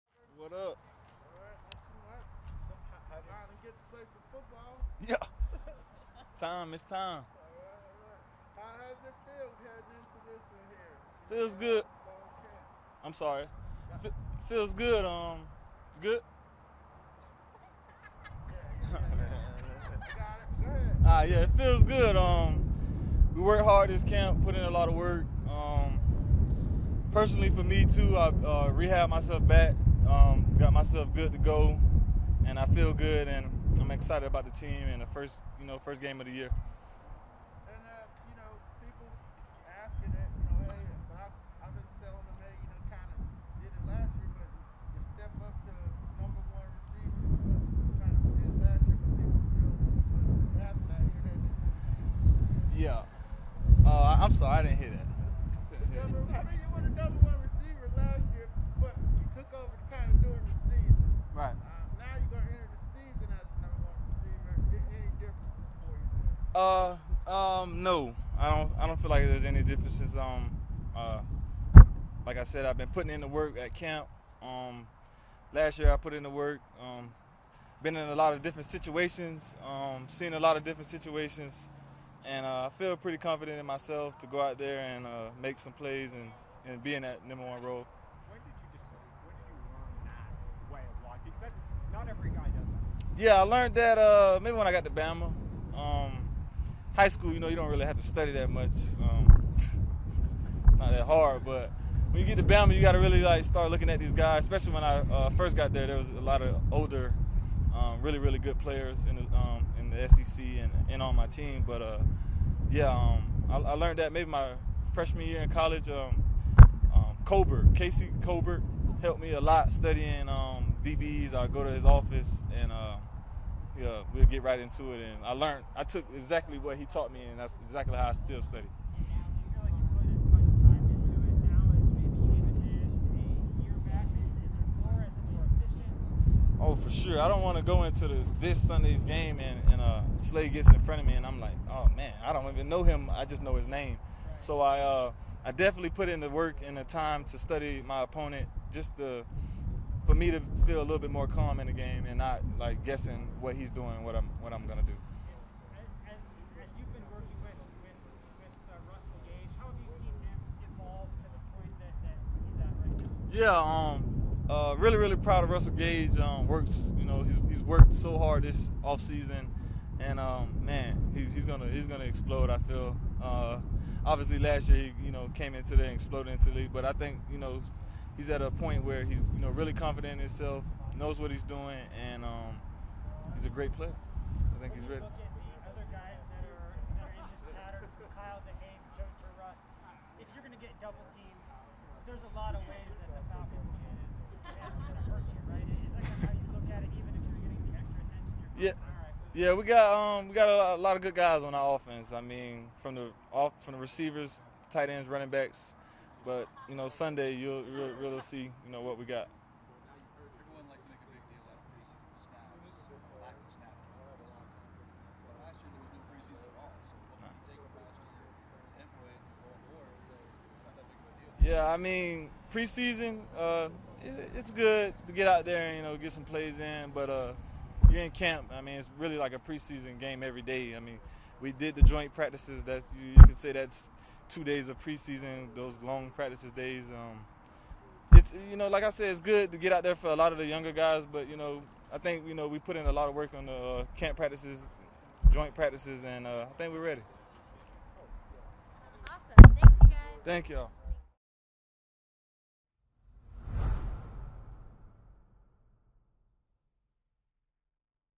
'I feel pretty confident in myself, being in that number one role' | Calvin Ridley Press Conference
Calvin Ridley speaks about being the number one receiver for the 2021 season and putting in the work to be prepared for the season.